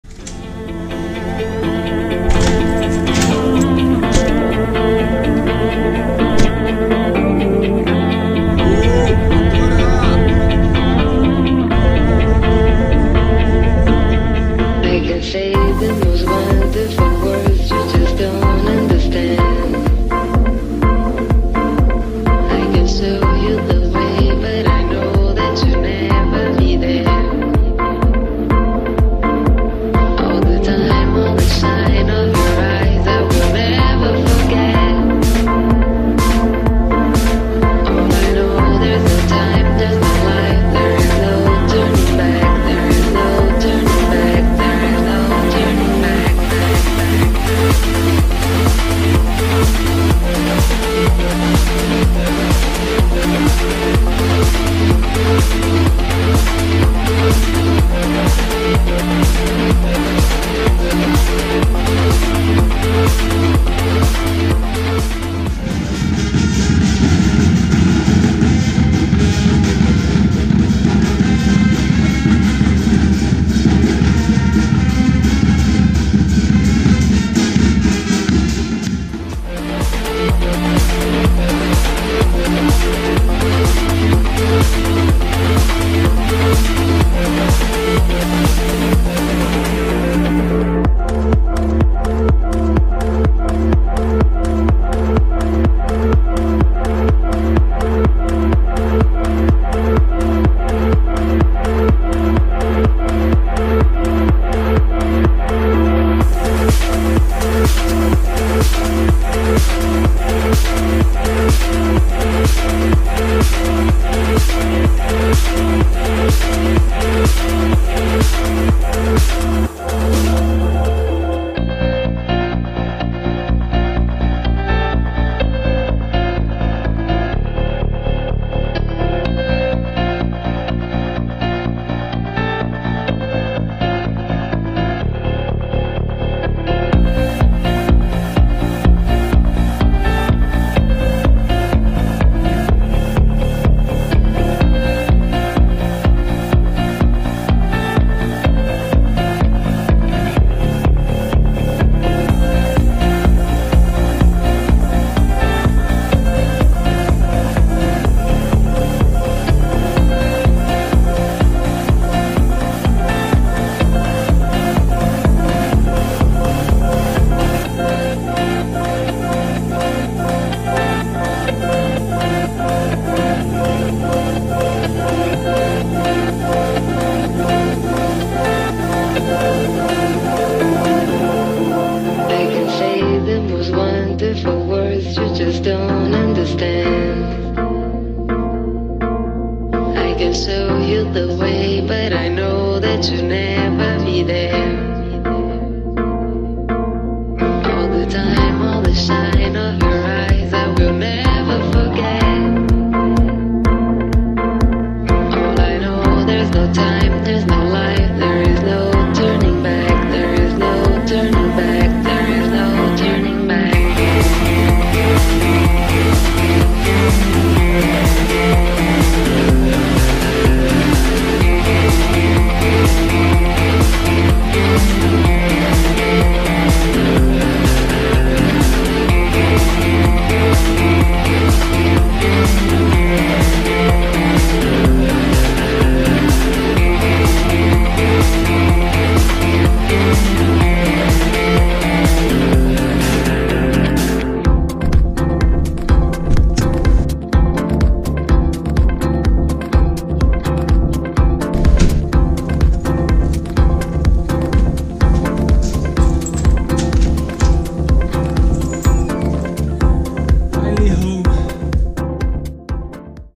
Weekend getaway to Andorra
A weekend trip from Barcelona to Andorra.